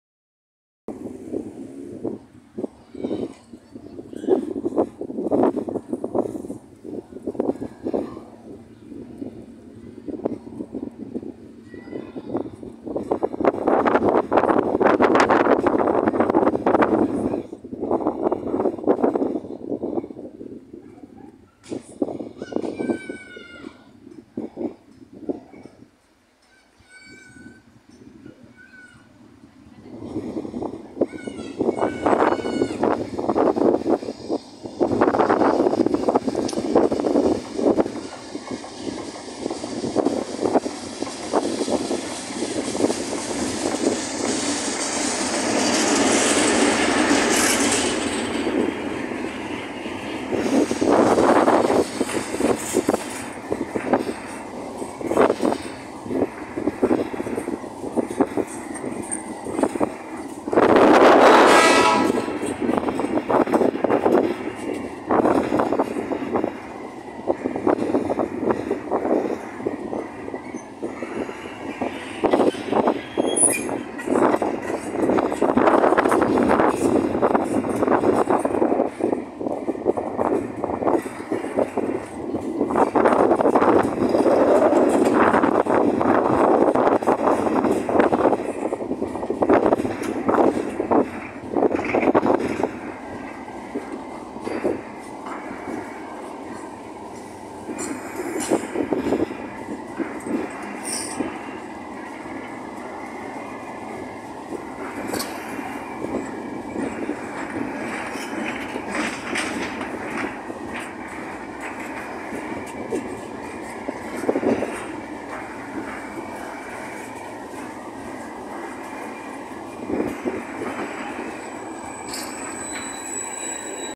GR12 6634 de NCA con Bobinero Vacío por Zeballos (06-09-2023)
En una tarde muy helada y ventosa, la GR12 6634 hace su pasada por Zeballos con el Bobinero Vacío hacía Gerli, proveniente de la planta SIDERAR, ubicada en Tolosa.
Filmado hoy 6 de septiembre 🎥